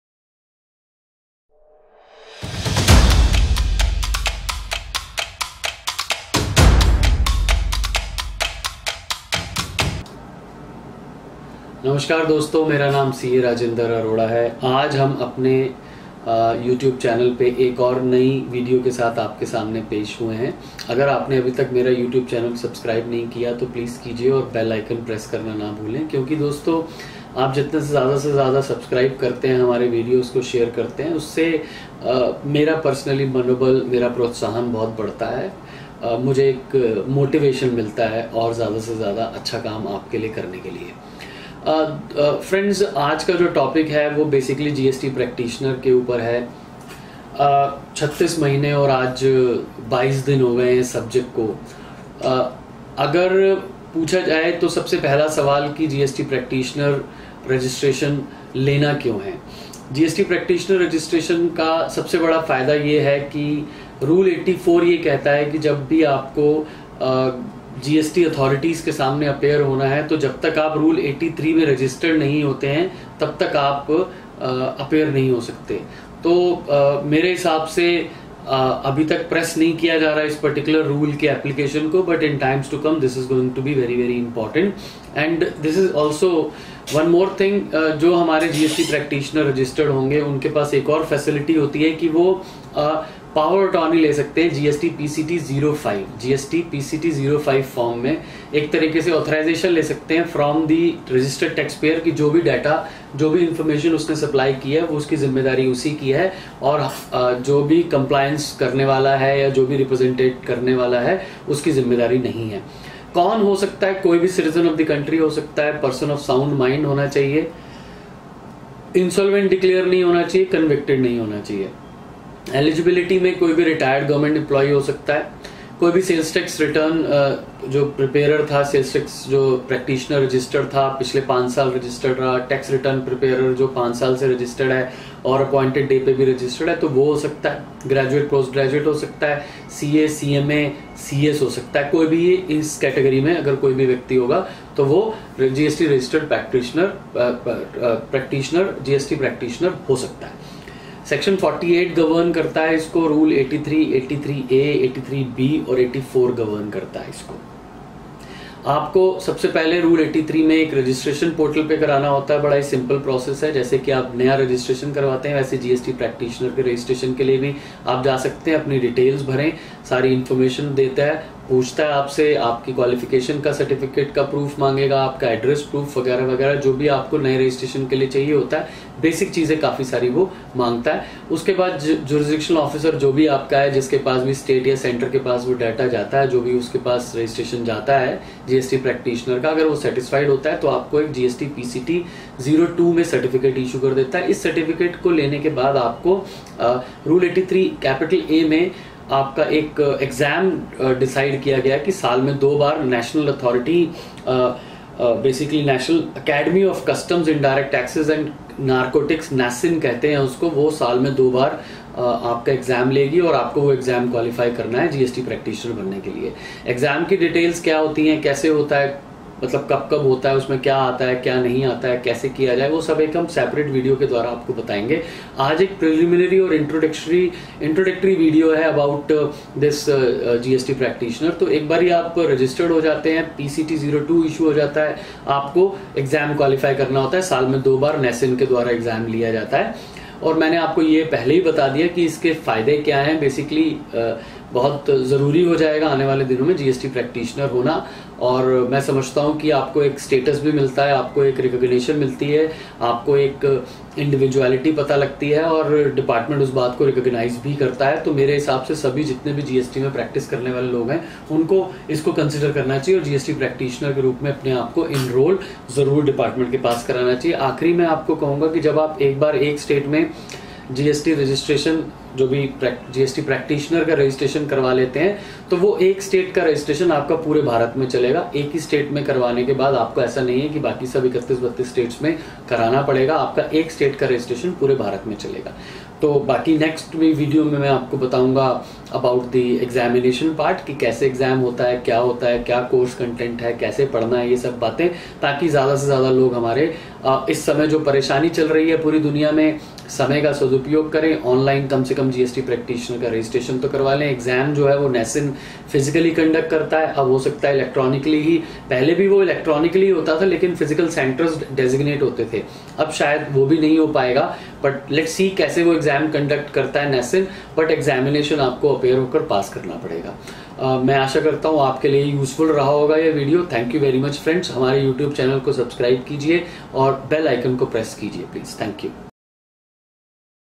GST Practitioner Benefits Role Eligibility with CA Rajender Arora
What is the benefit of getting enrolled for GST Practitioner?

Who can be GST Practitioner?

How to become GST Practitioner?

How to qualify GST Practitioner Exam?